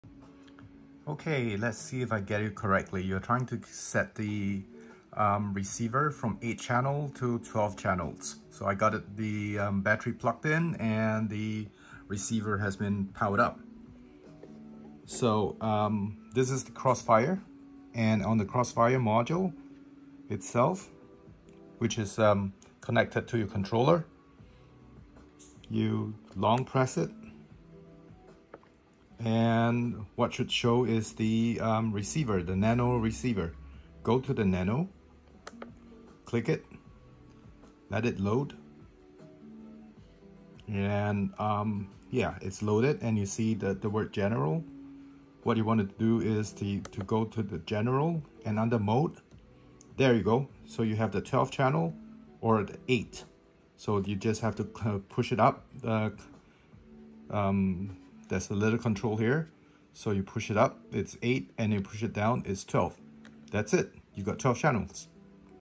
FPV -- How to switch TBS Nano Receiver from 8 channels to 12
Just a short video to explain how the switch from 8 channels to 12 is done.